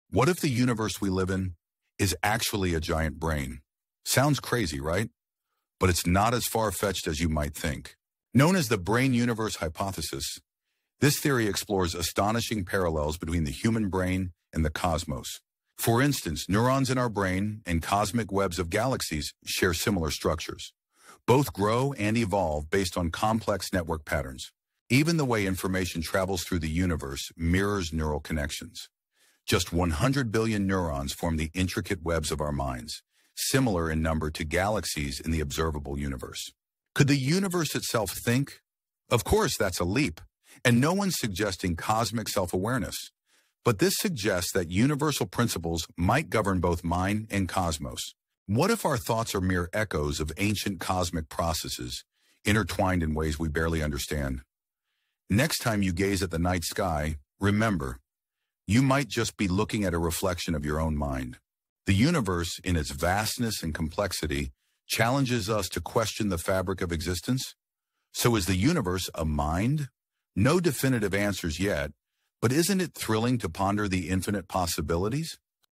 What if the universe itself is a giant brain?
What if the universe itself is a giant brain? 🤯 The Brain Universe Hypothesis explores eerie similarities between the structure of the cosmos and the human mind. 🌌🧠 Just as neurons form intricate networks in our brains, galaxies are linked in vast cosmic webs. Even the way information flows in both systems follows strikingly similar patterns. Could universal principles connect mind and matter in ways we barely understand? While we’re not saying the universe is self-aware, it does raise fascinating questions about the nature of reality. 🌀✨Next time you look at the stars, remember—you might just be staring at the reflection of your own mind. 🌠💭📌 Watch till the end & let us know—what do you think? Is the universe more than just space and time? 👇Hashtags:Science